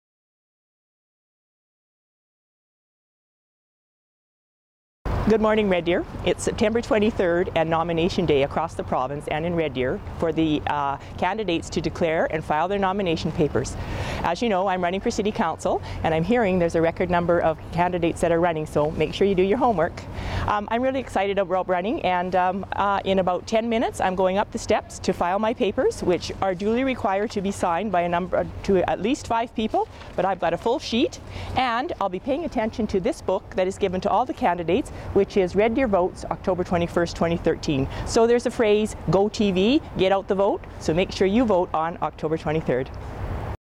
Dianne - running for public office and importance of elections
On September 23, Dianne Wyntjes officially filed her nomination papers for entering the race for Red Deer City Council, an important day for democracy in action in Red Deer.